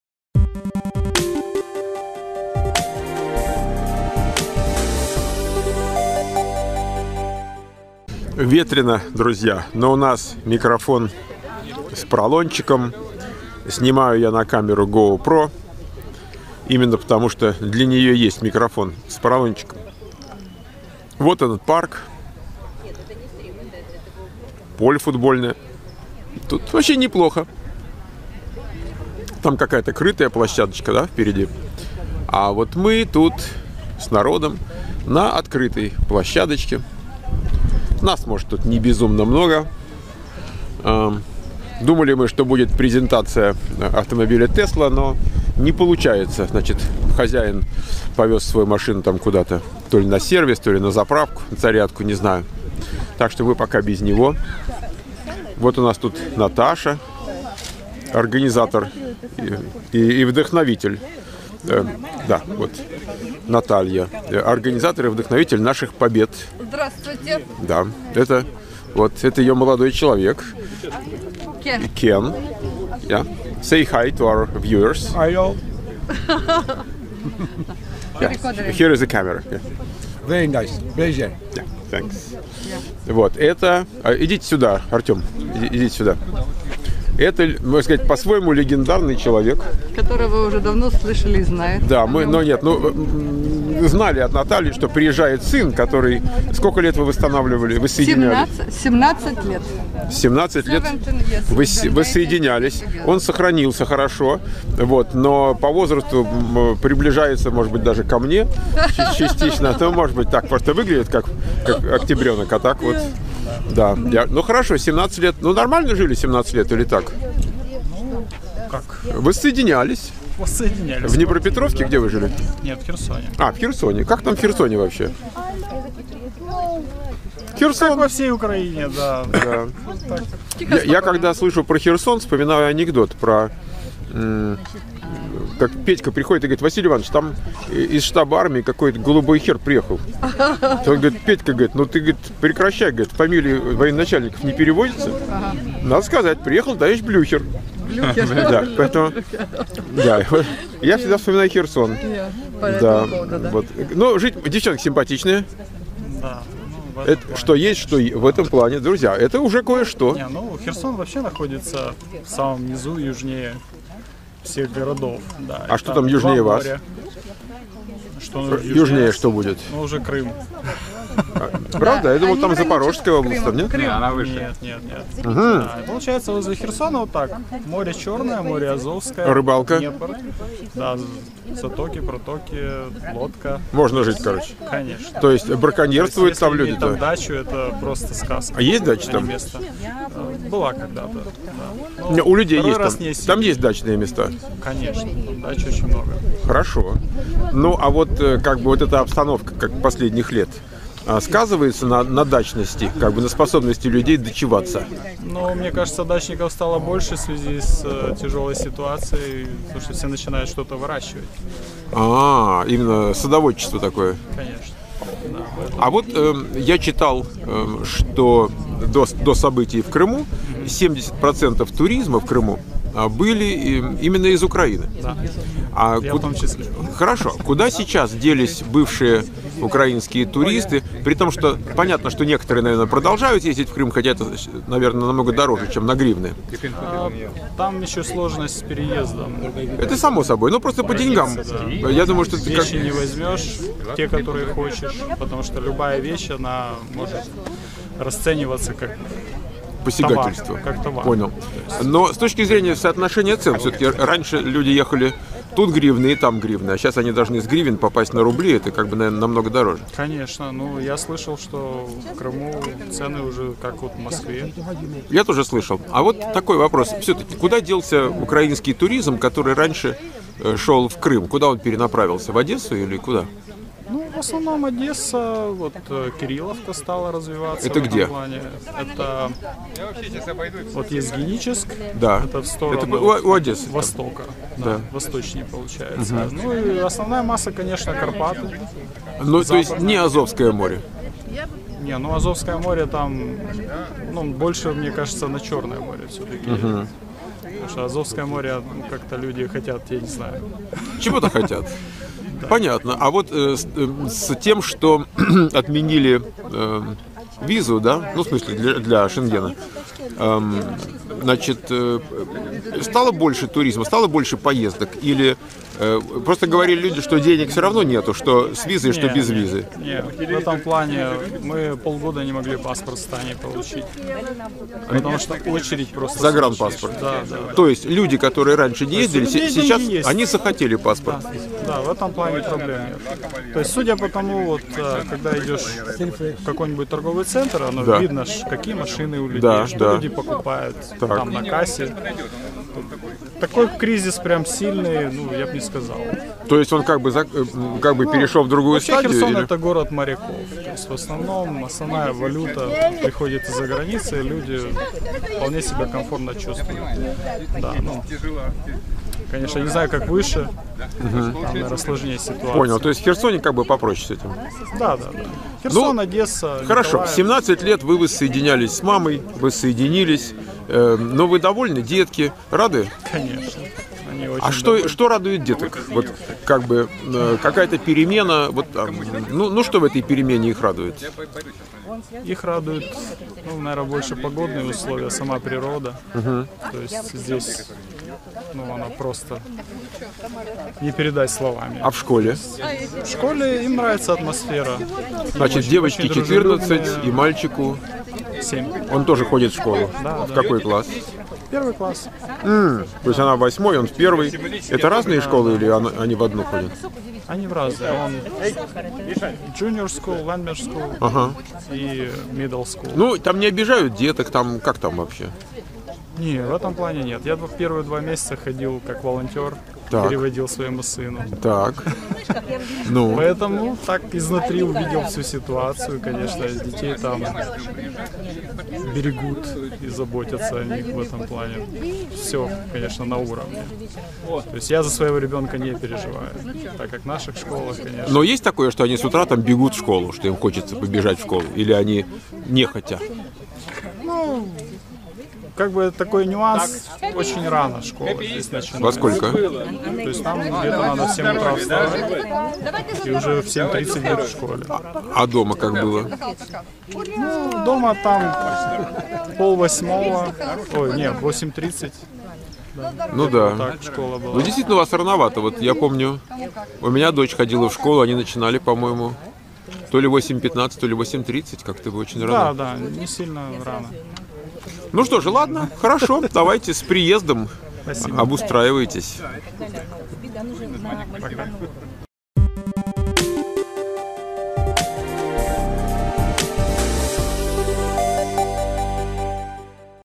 США 5173: Новички из Херсона - 17 лет на воссоединение сына с его семьей и матери - гражданки США
Новички из Херсона - 17 лет на воссоединение сына с его семьей и матери - гражданки США
Ссылки на канале SiliconValleyVoice /школа Михаил Портнов: